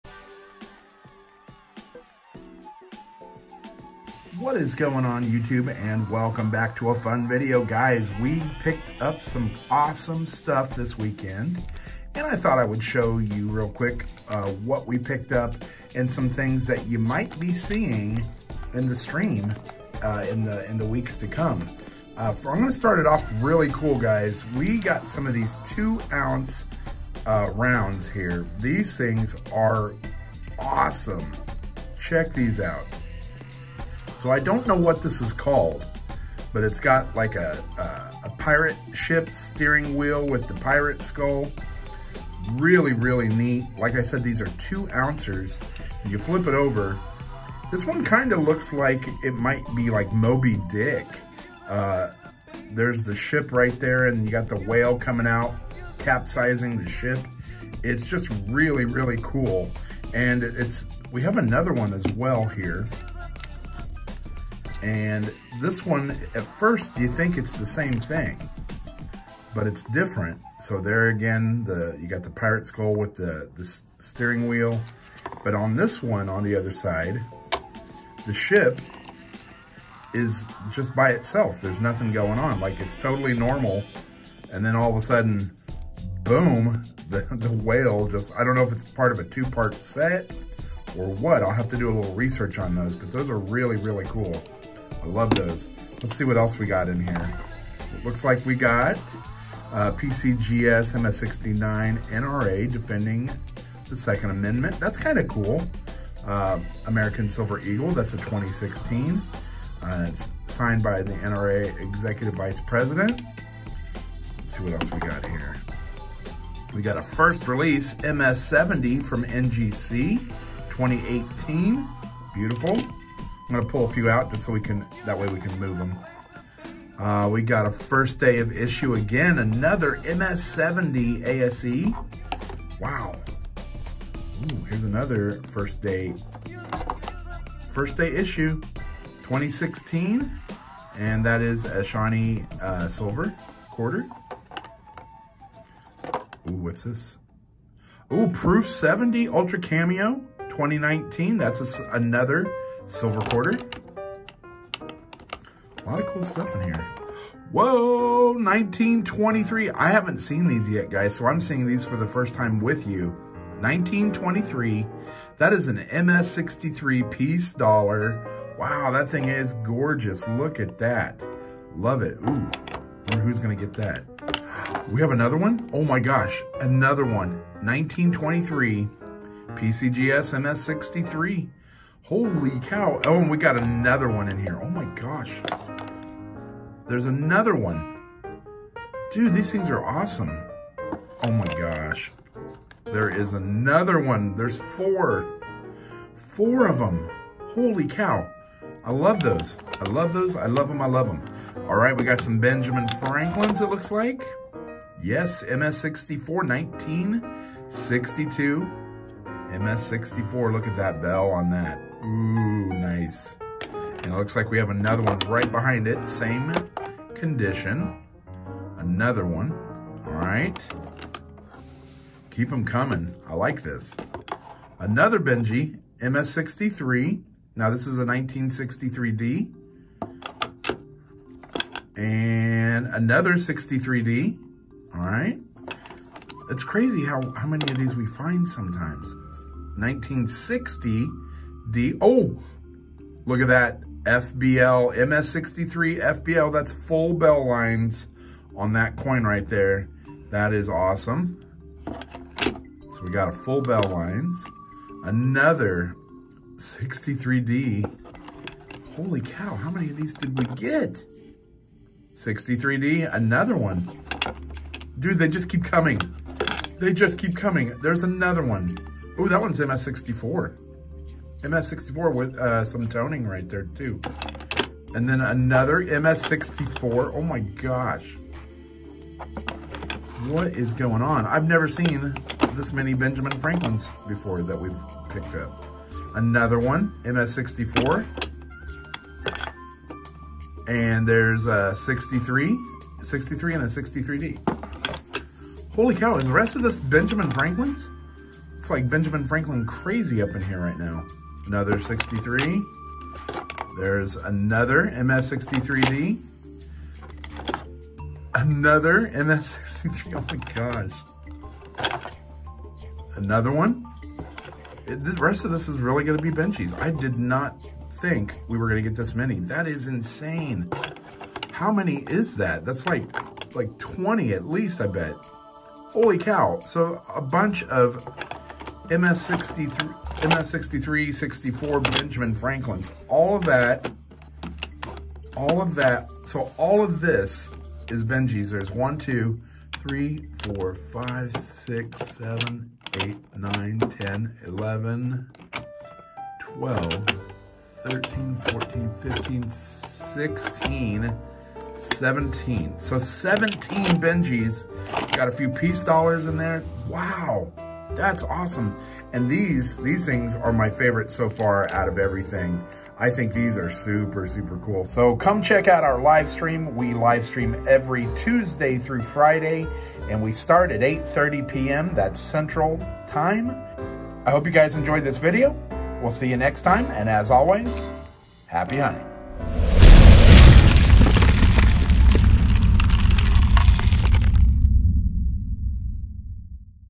Pirate Rounds! Look What We Picked Up!
Lot of pick ups this weekend including some awesome 2 ounce pirate rounds!